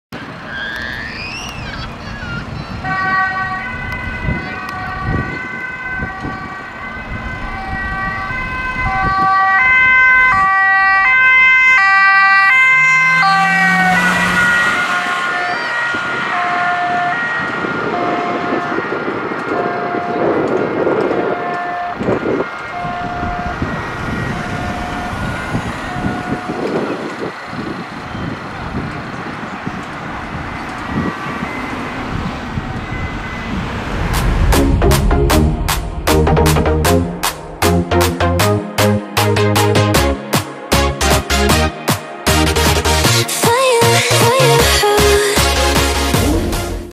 A1 CvD Rav 18-550 naar Gorinchem vanaf Opendag ADZHZ Post Papendrecht
Video: Jeffrey Cleton

De Cvd-rav 18-550 vertrekt met spoed vanaf de opendag van de ambulancepost Vliedberg in papendrecht 20 april 2019 naar een mmt inzet in JHR MR AF De Savornin Lohmanstraat in Gorinchem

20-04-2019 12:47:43
A1 (DIA: ) AMBU 18550 HR.MR. A.F. DE SAVORNIN LOHMANSTRAAT GORINCHEM bon 52053